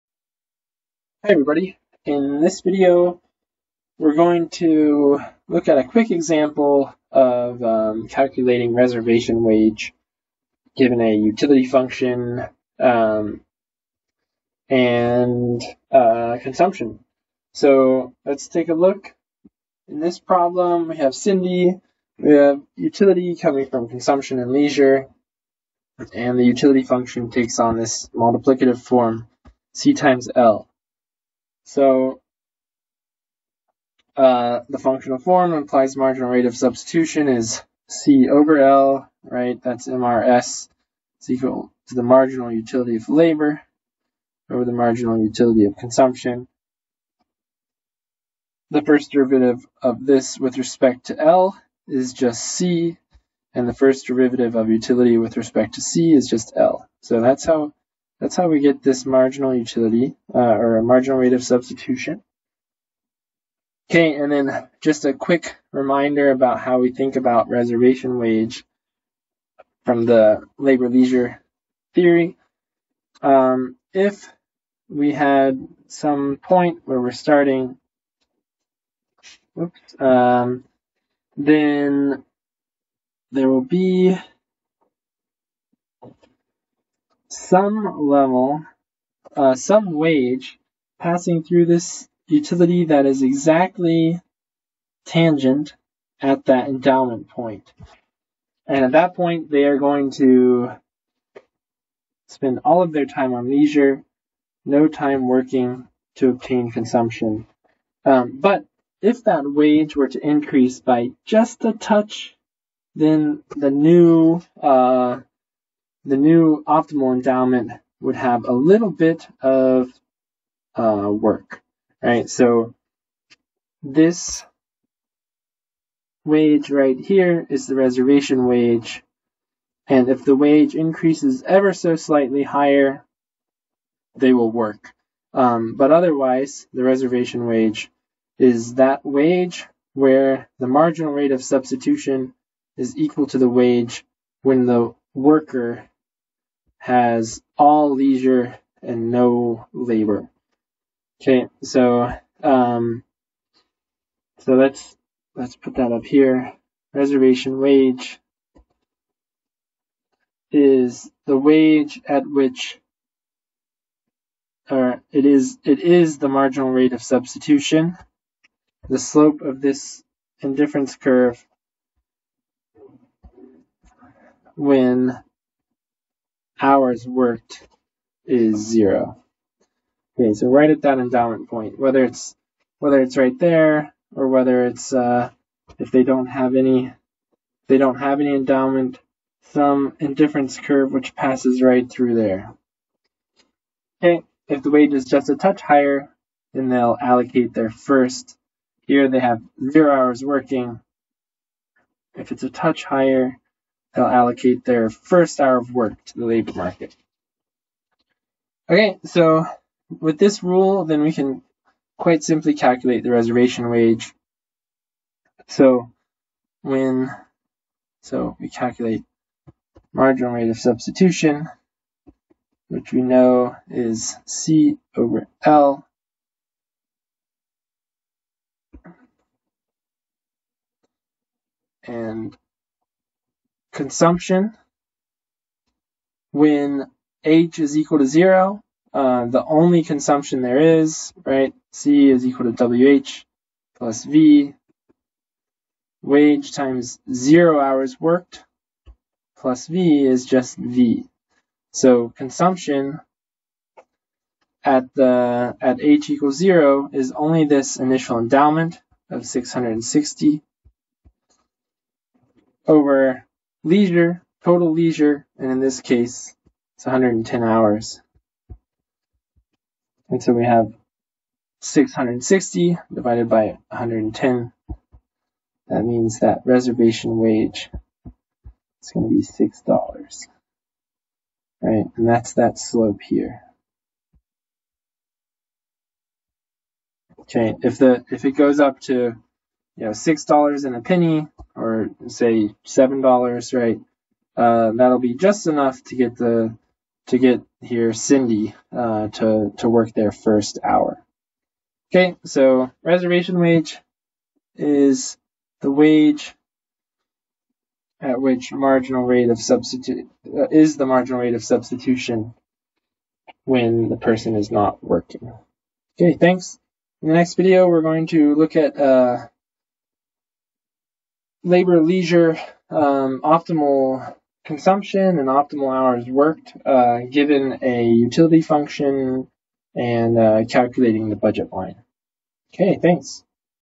Reservation Wage Calculation
Calculating the reservation in the model of labor-leisure choice.
Reservation wage is equal to the Marginal Rate of Substitution (MRS) when hours worked, h, equals zero.

Borjas - Labor Economics - Chapter 2